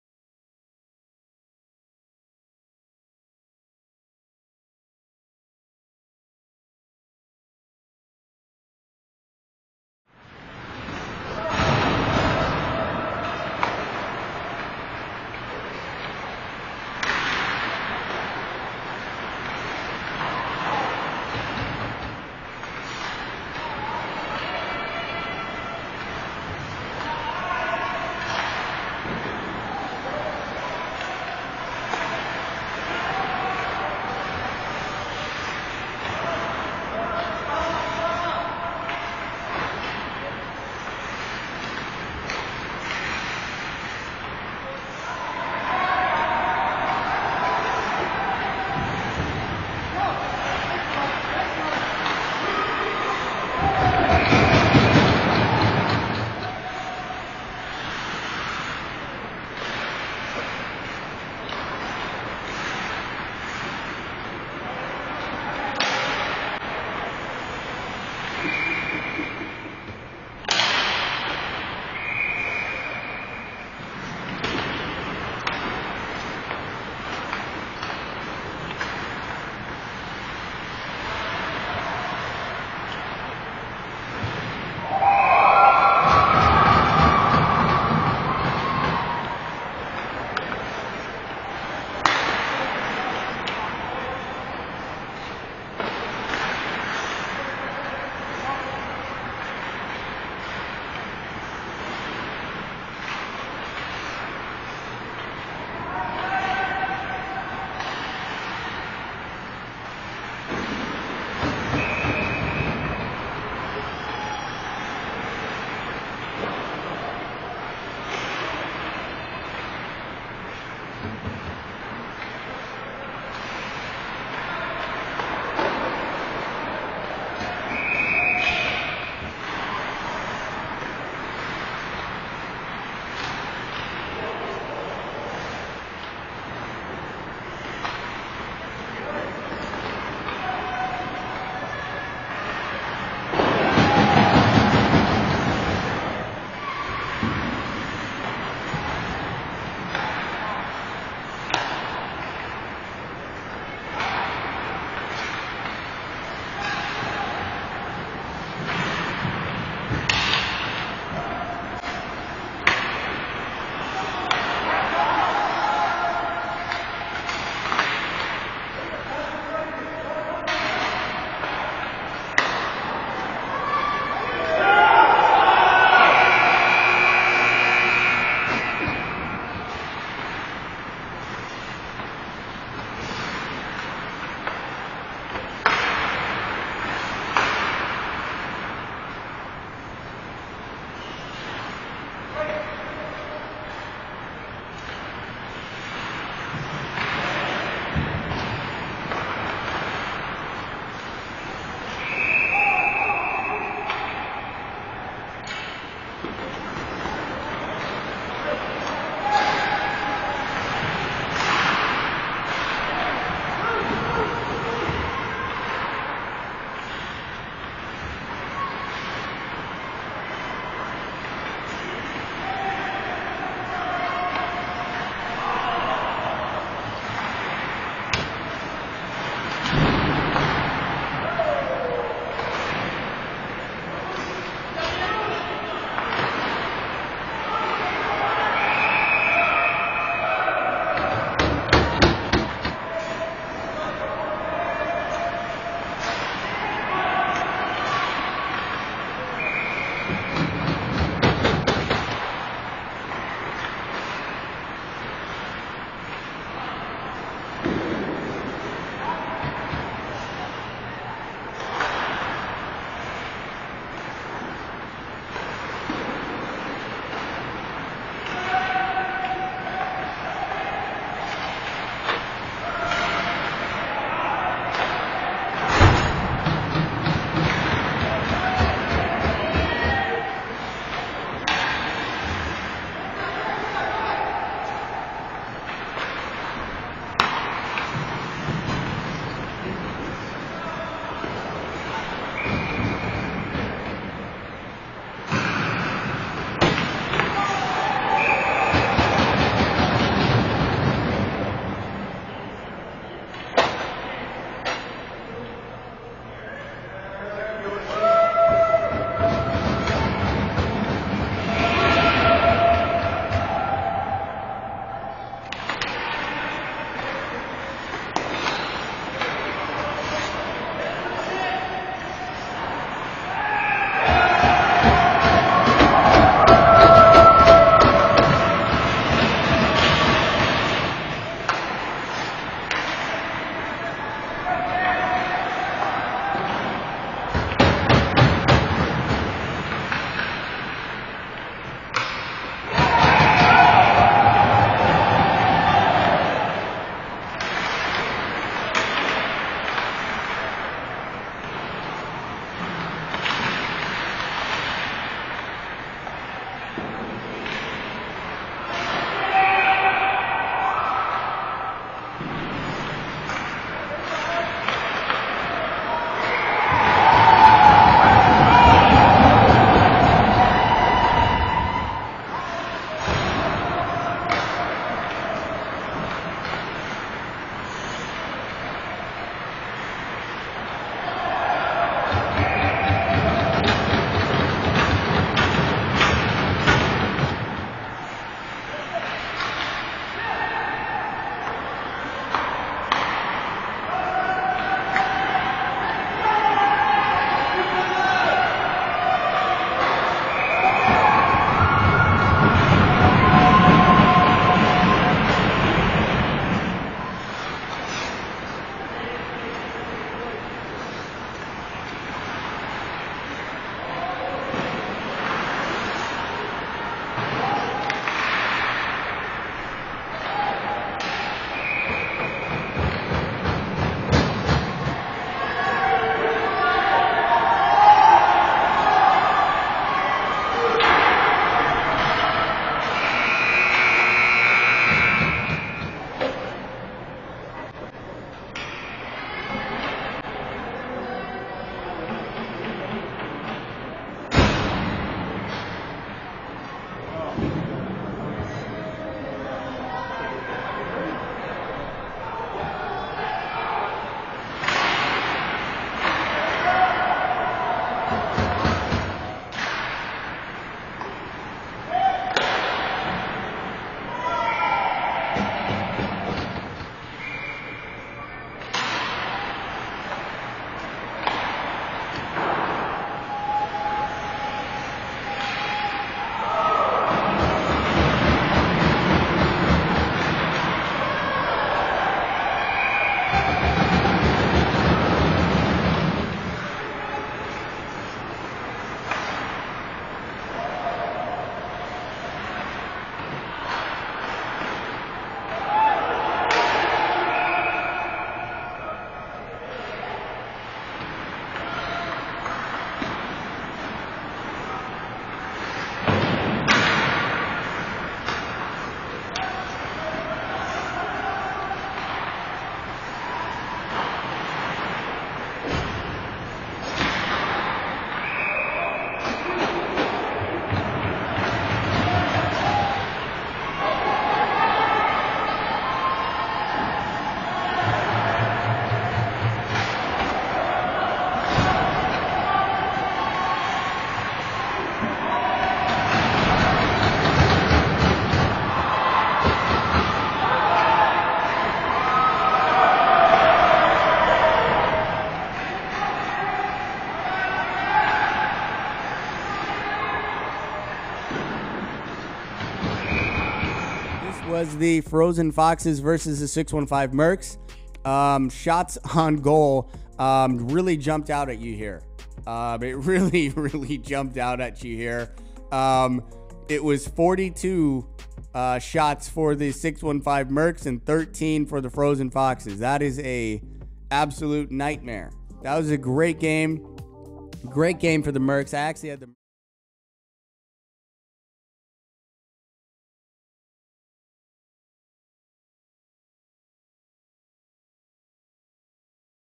615 MERCS vs. Frozen Foxes Highlights
615 MERCS vs. Frozen Foxes
June 9, 2021
Ford Ice Center Bellevue
Adult Rookie League
Summer 2021